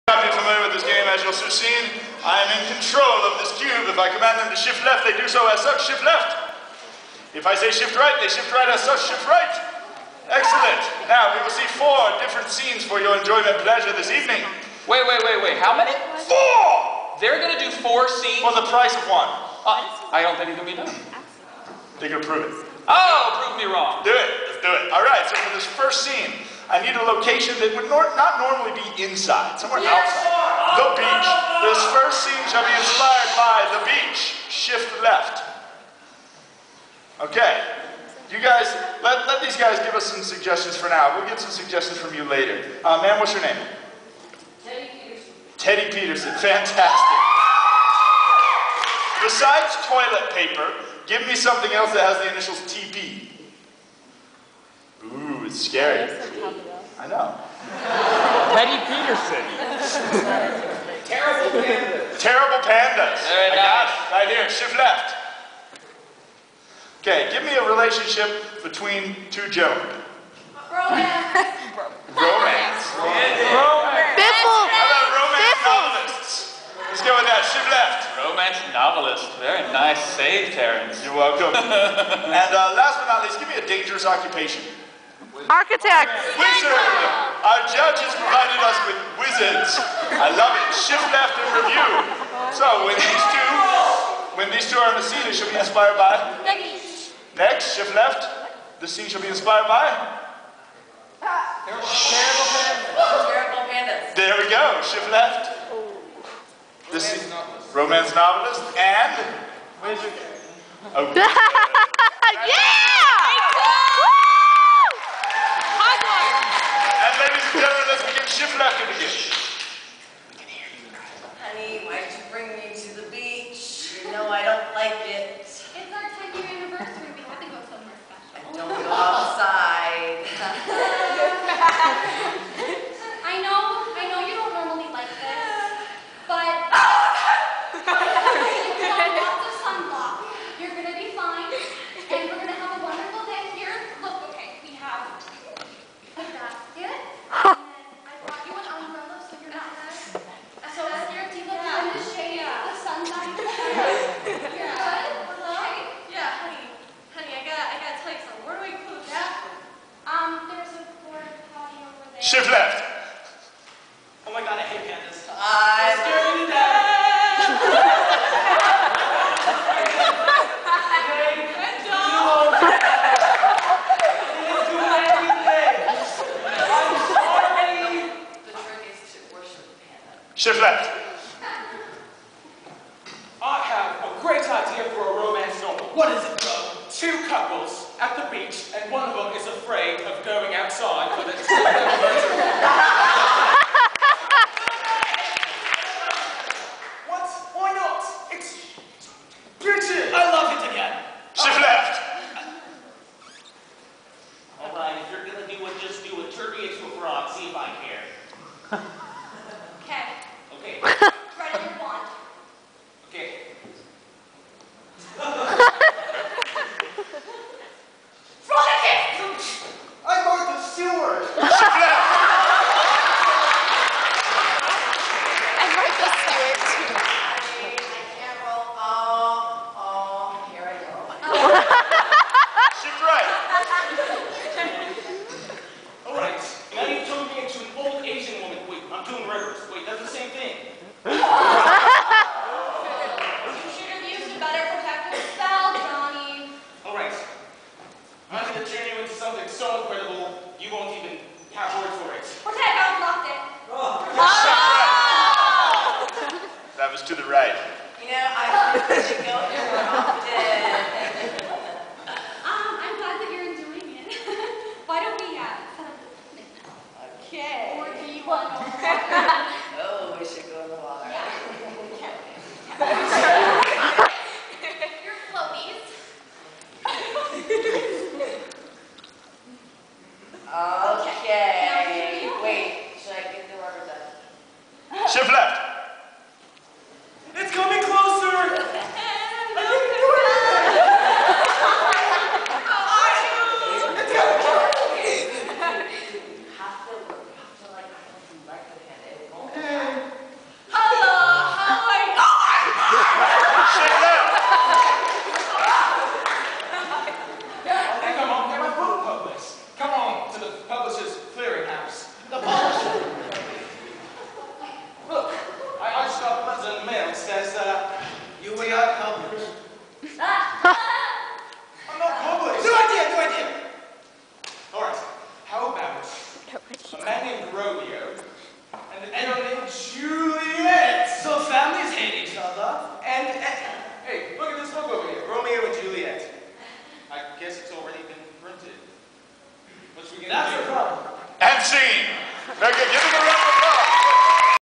Improv Festival Competition (part 2)
The Jets (light blue team) playing Switch Left :)